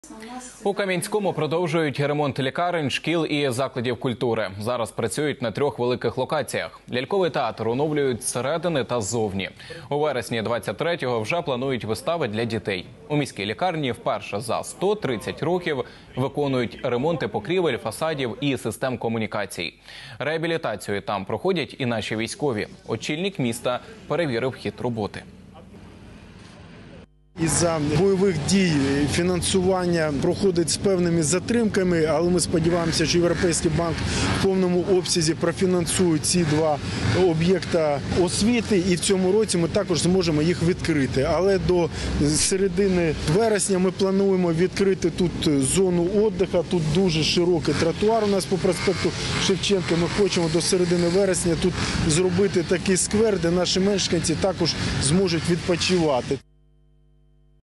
У Кам'янському продовжують ремонт лікарень, шкіл і закладів культури
У Кам'янському продовжують ремонт лікарень, шкіл і закладів культури. Зараз працюють на трьох великих локаціях. Ляльковий театр оновлюють зсередини та ззовні. У вересні 2023-го вже планують вистави для дітей. У міській лікарні вперше за 130 років виконують ремонти покрівель, фасадів і систем комунікацій.